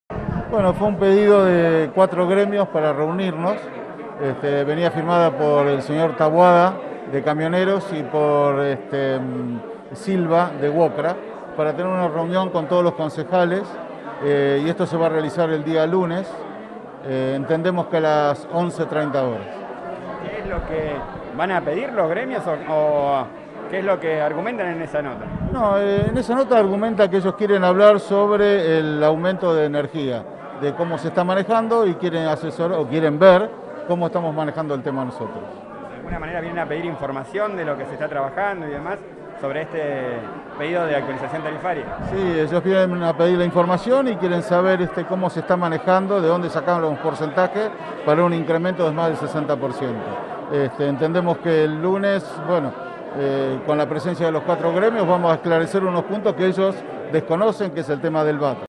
Gremios pidieron reunión en concejales por readecuación tarifaría
Gremios del Petroleo, la construcción y de camioneros, pidieron una reunión con los concejales de los distintos bloques para el día lunes a la mañana en referencia al pedido de readecuación tarifaría que solicita la Sociedad Cooperativa Popular Limitada.